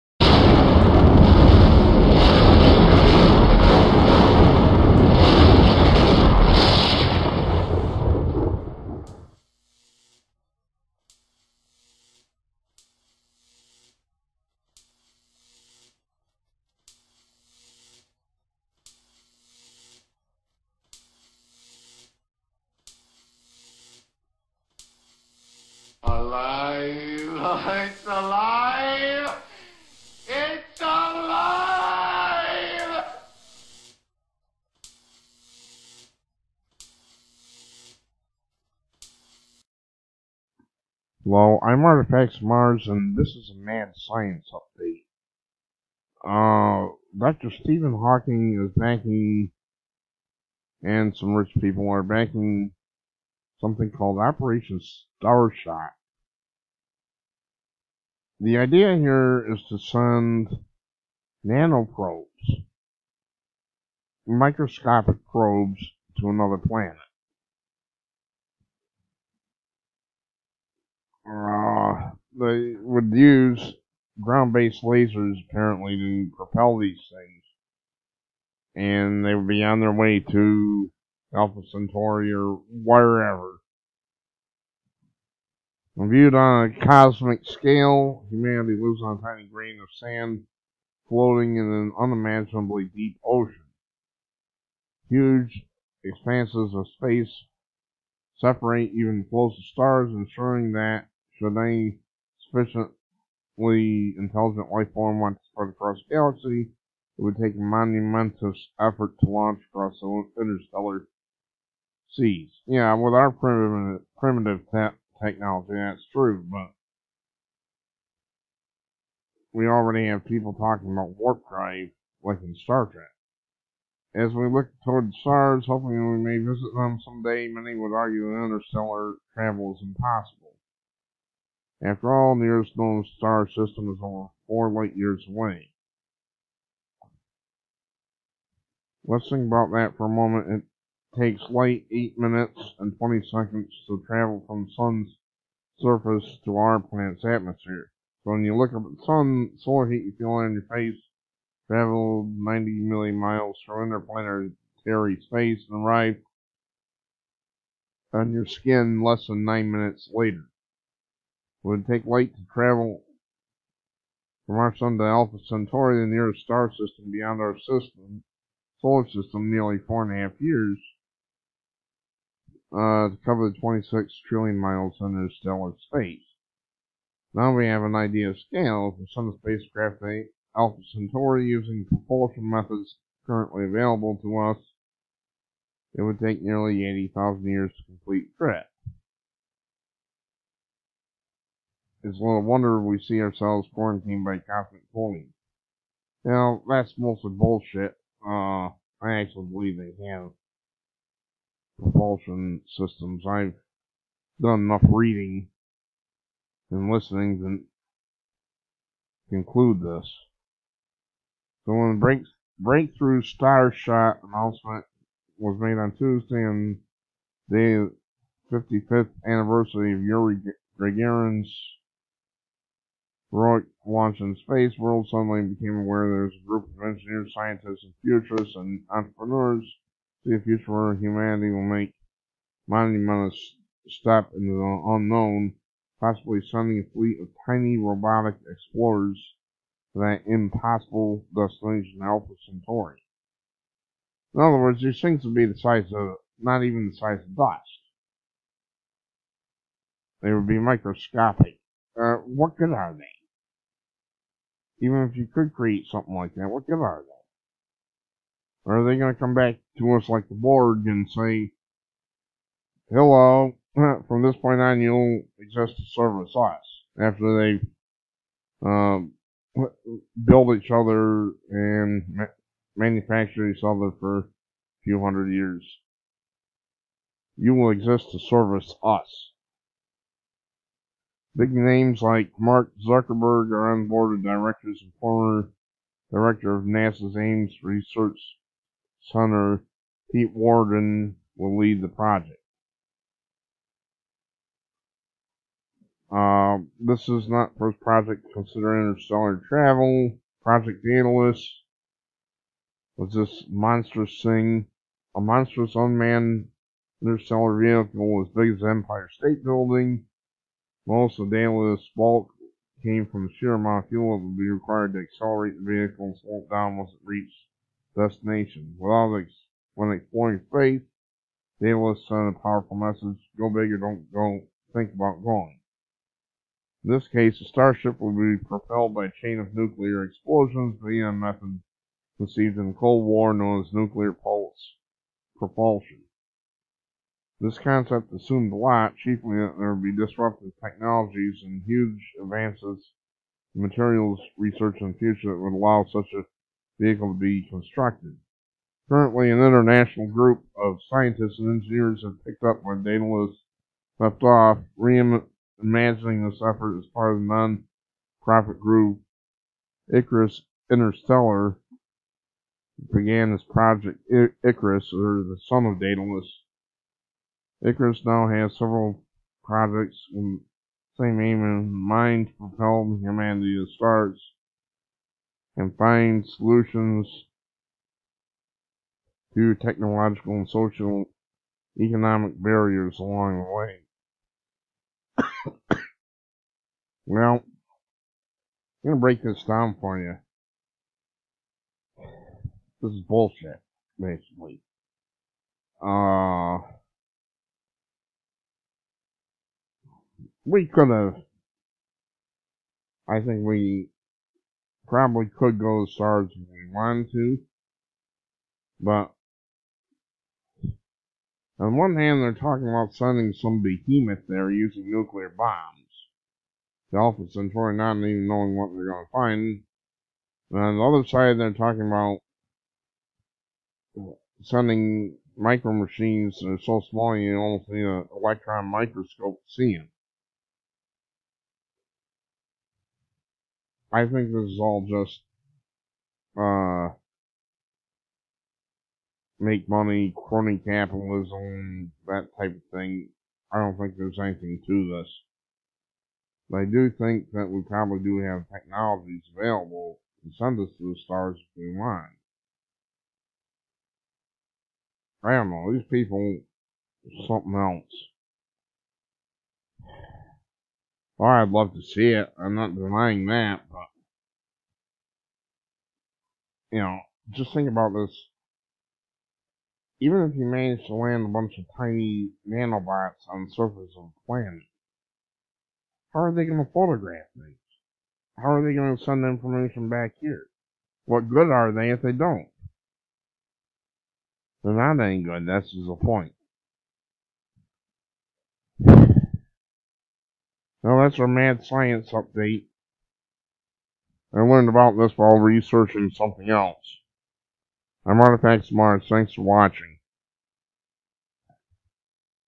Scientists want to send nanobots to distant planets as project Starshot
In a bizarre project, Dr. Stephen Hawking, a Russian billionaire and other elitists want to send microscopic(or at least very tiny)  machines called Nanobots to Aloha Centauri or other systems by using lasers to propel them.
     I ask how these things could even enter the atmosphere and how they could beam information back to the earth. The article is a little vague on specifics. Go figure.